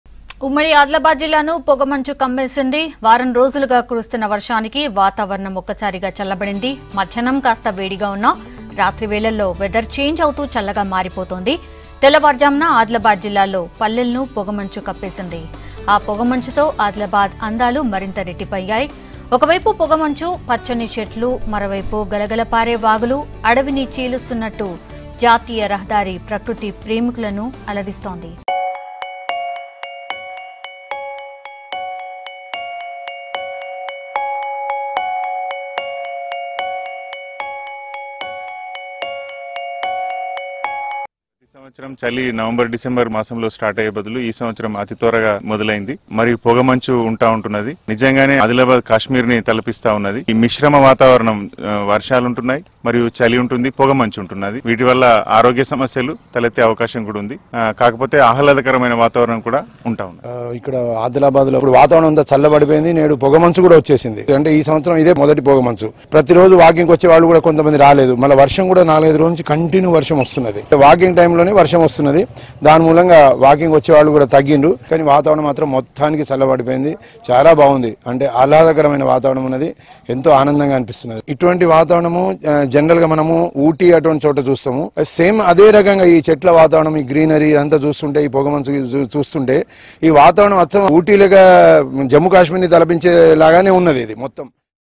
Fog A Rarity This Season With Heavy Rain An Adilabad | V6 Telugu News
Fog A Rarity This Season With Heavy Rain An Adilabad

బంగారు మంత్రులు జోష్... ఉద్యమ మంత్రులు కామూష్ |